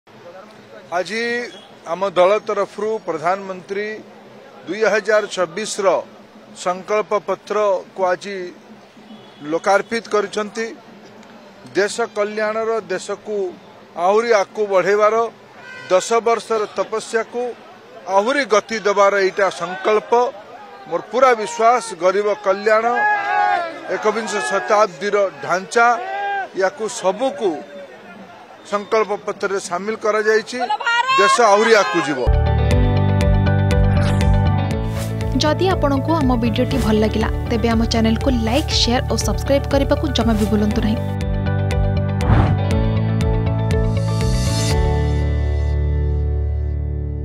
୨୪ ନିର୍ବାଚନ ପାଇଁ ବିଜେପିର `ସଂକଳ୍ପ ପତ୍ର' || Dharmendra Pradhan
`ସଂକଳ୍ପ ପତ୍ର' ଲୋକାର୍ପଣ କଲେ ପ୍ରଧାନମନ୍ତ୍ରୀ ନରେନ୍ଦ୍ର ମୋଦି ।